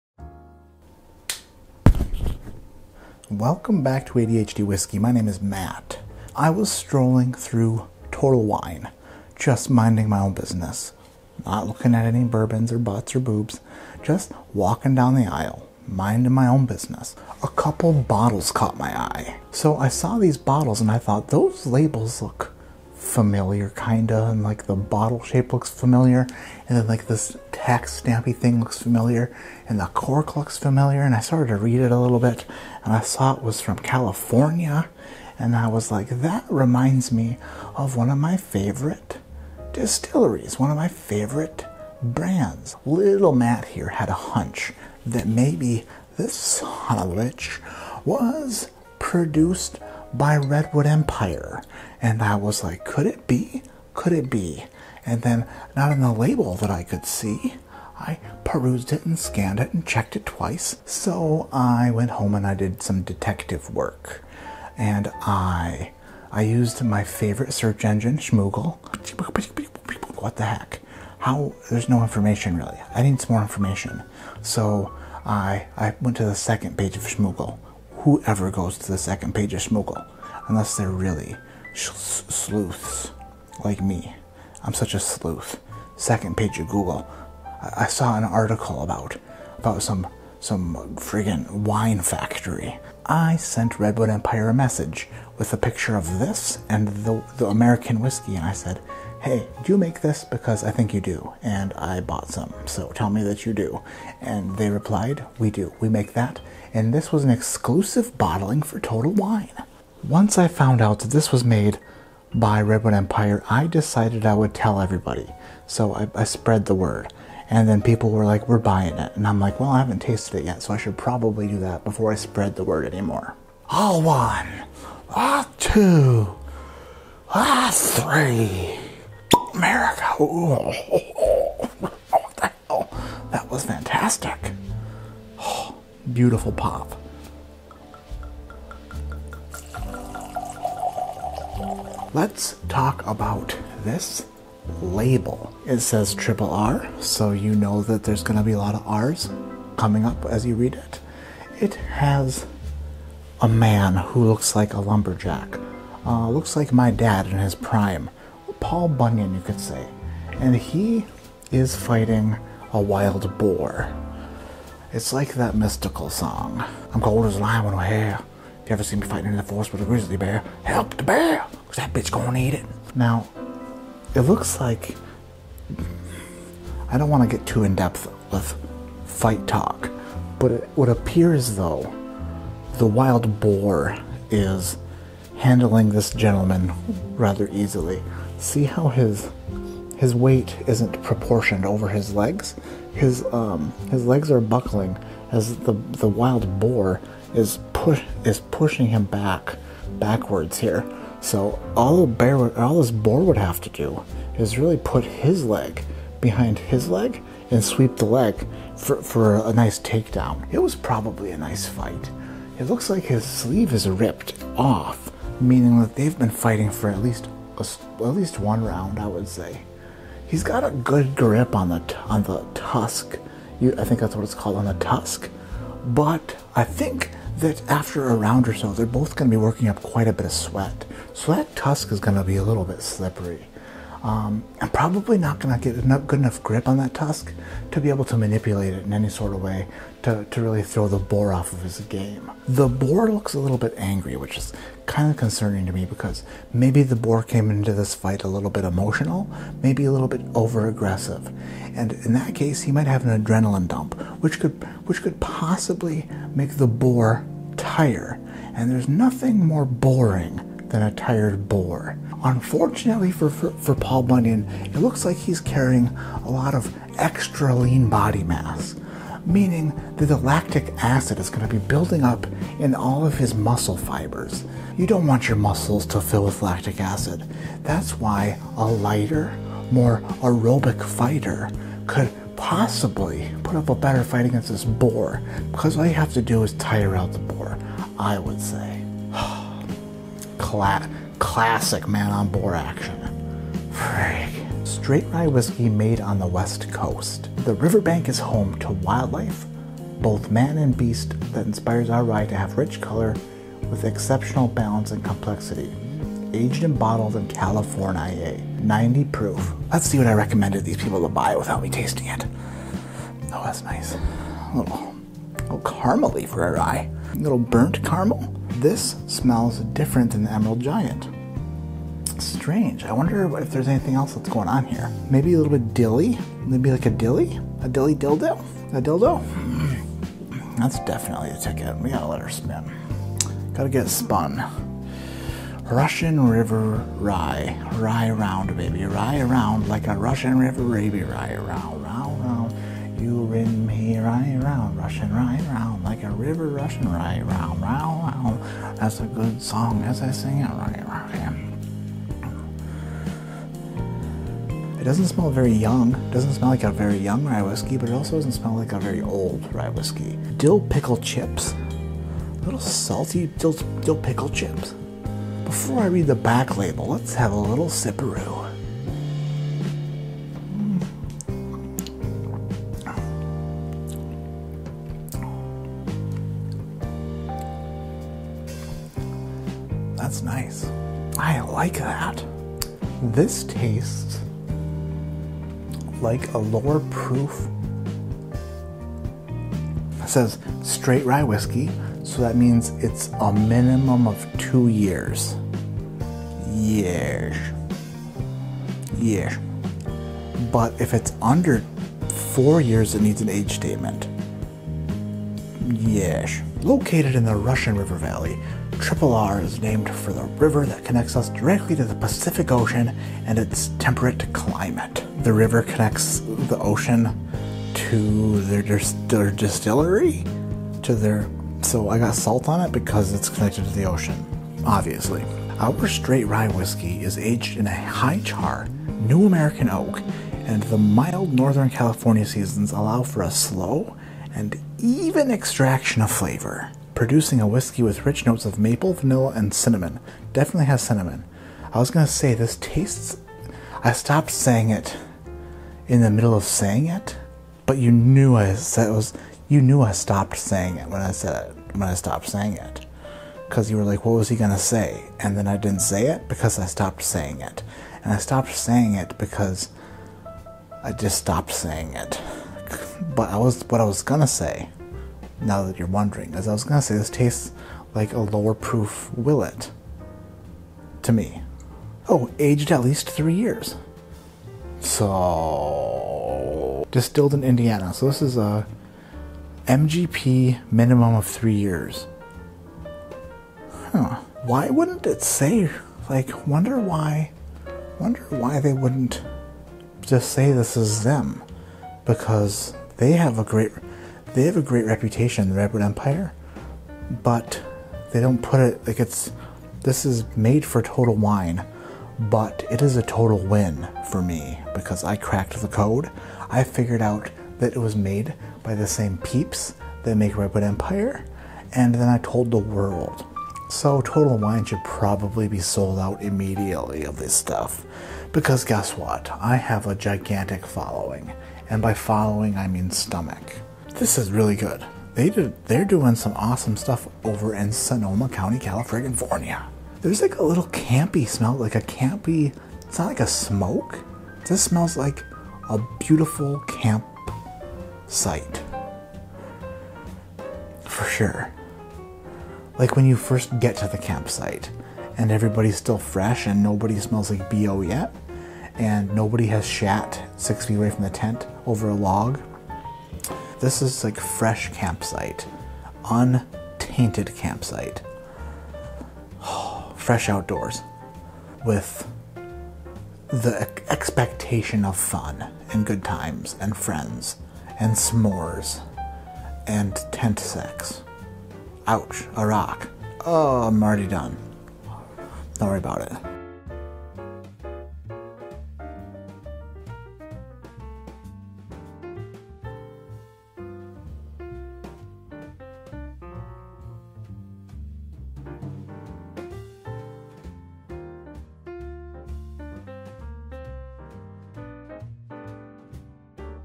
What is an Exclusive Bottling?? Russian River Rye Whiskey
What is an exclusive bottling? Let's look into that! This Russian River Rye was exclusively bottled for Total Wine by Redwood Empire...but you'd never know by reading the label.

⬇️MERCH⬇️

Include Your USPS Mailing Address!

CHALLENGE COINS!

TEE SHIRTS!